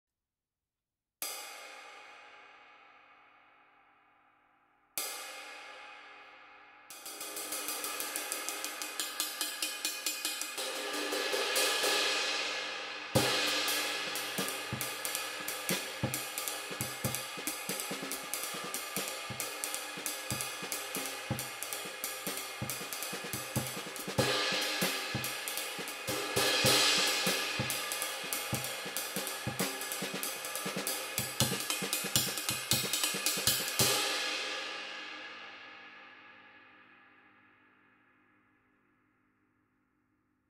21 Rustico Ride, 2034g - Handcrafted cymbals by Craig Lauritsen
Hammered from blank to final shape in one session and then sparingly lathed.